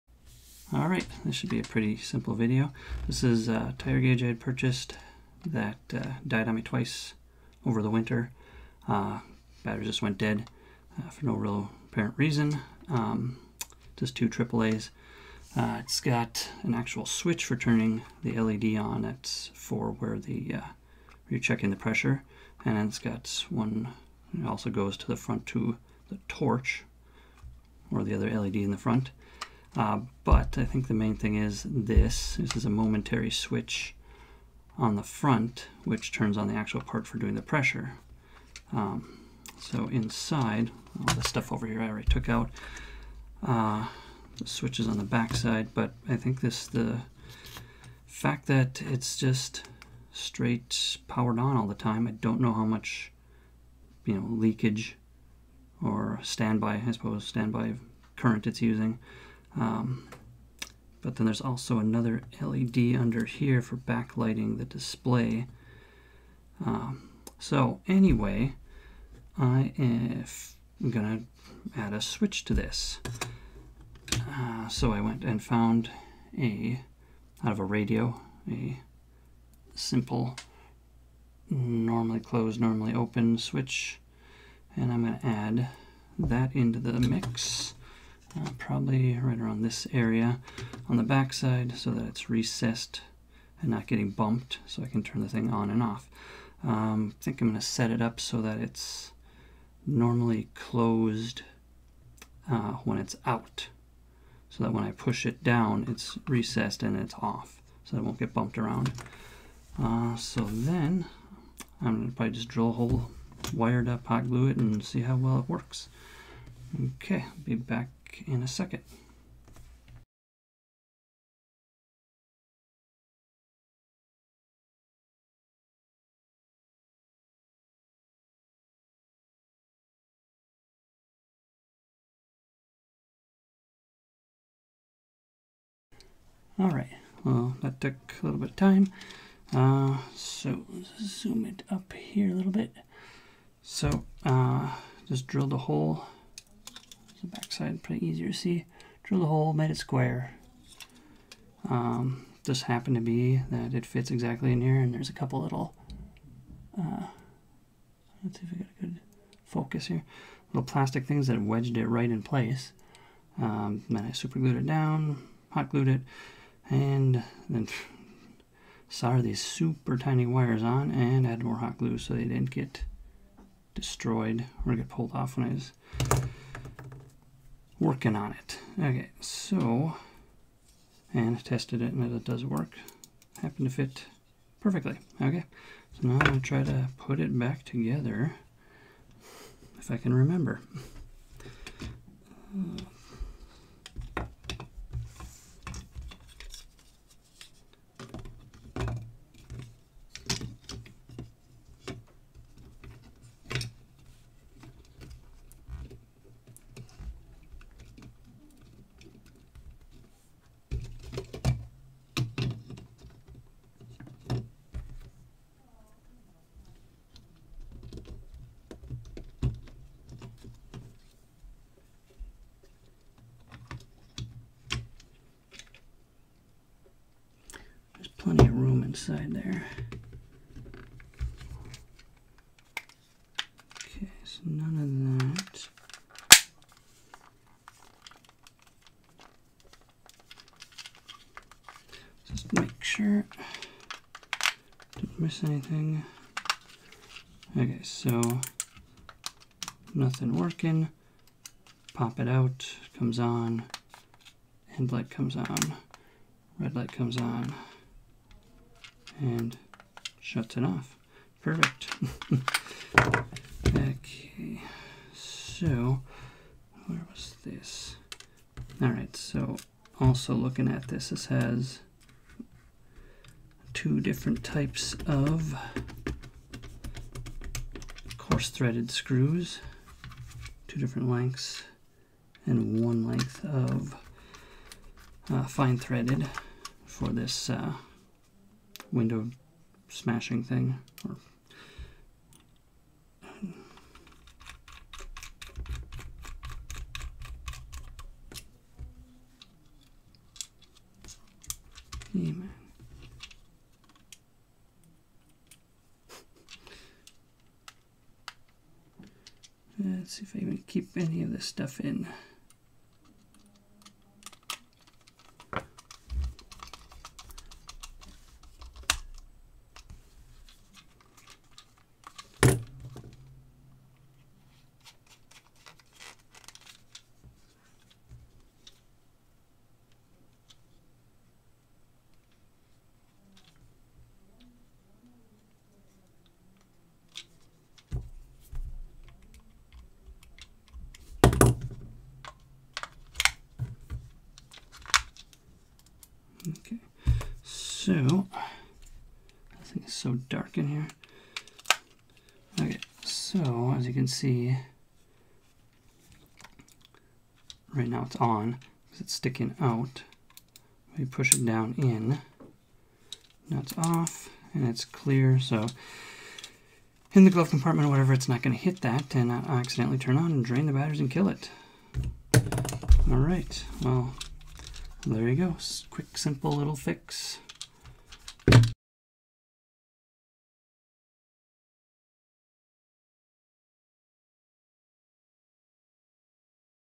Fixing Digital Tire Pressure Gauge Battery Drain
Adding an off switch to a digital tire pressure gauge to keep the battery from draining.

The tire gauge:
Described as:Sivton Lantoo Digital Tire Pressure Gauge, 150PSI with 5 in 1 Rescue Tools of LED Flashlight,Car Window Breaker, Seatbelt Cutter,Red Safety Light and Tire Gauge. ST-A0012

Video Recording: Canon EOS M6 Mark II
Audio Recording: Blue YETI Pro and Audacity
Video Editing: DaVinci Resolve